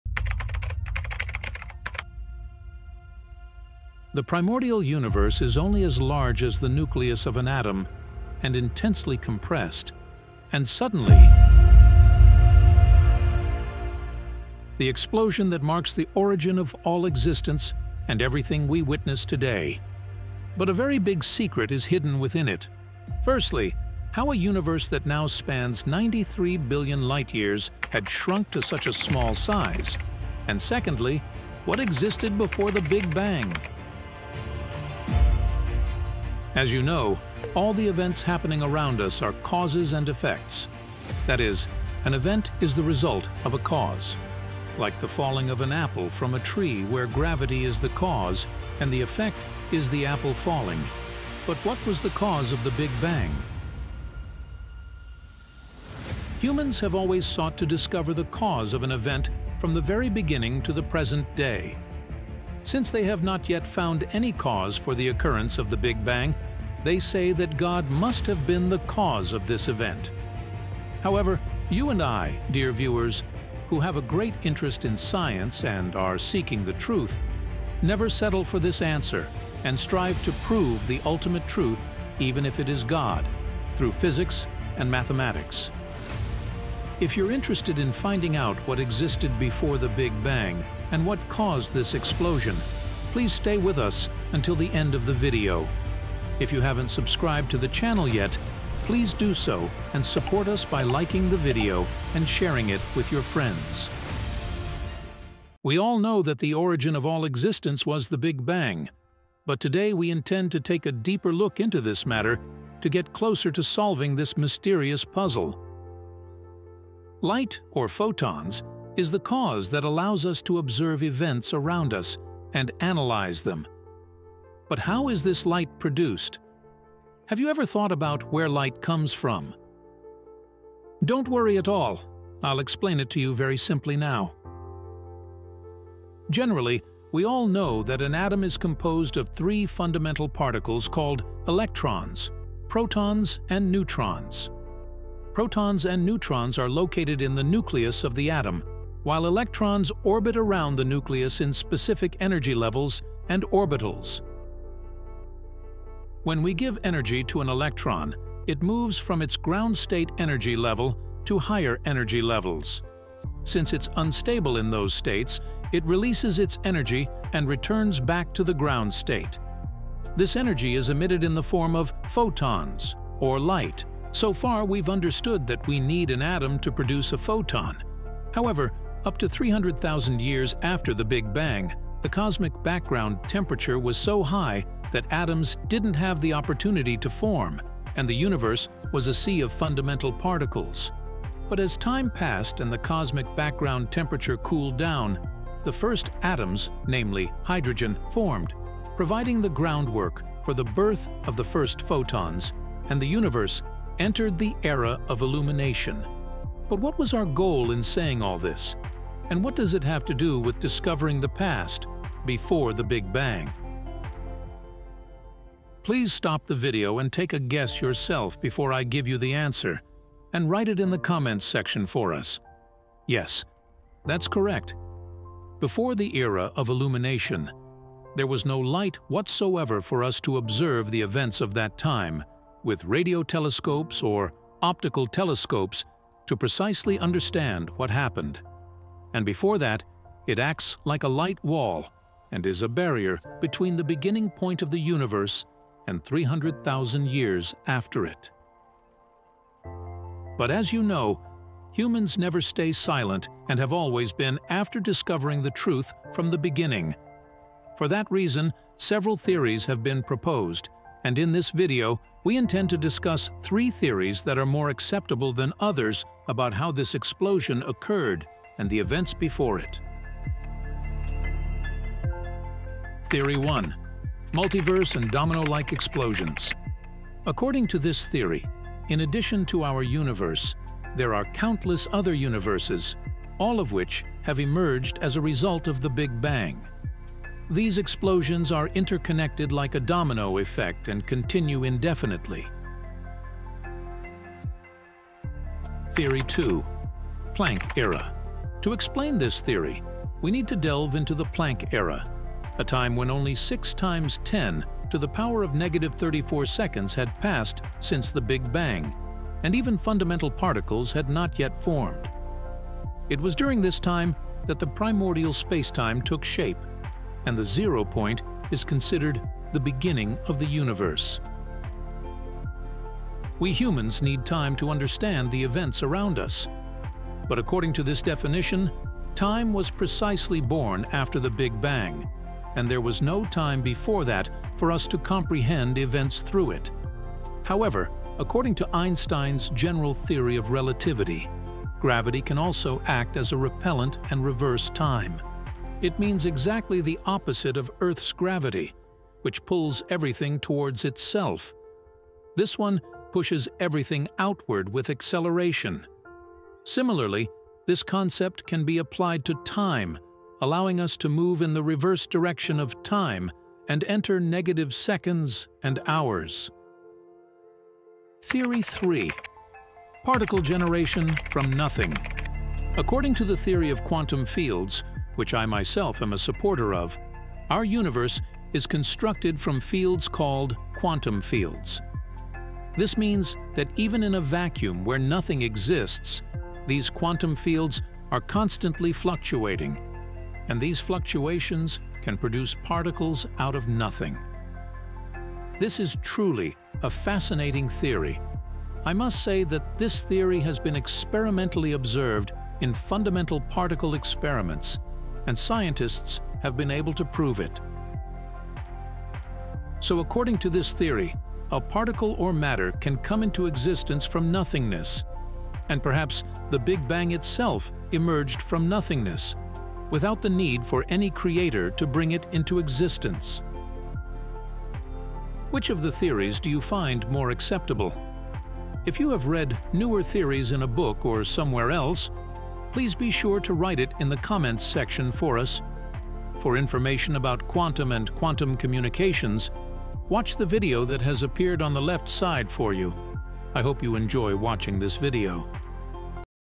why we cant see before bigbang?A universe that came out of nothing.
In this video, we have discussed a very big secret about the universe. What happened before the Big Bang and was there another universe before us?
First, we have fully explained one of the reasons that we cannot see before the Big Bang, and finally, three theories that explain what happened before the Big Bang.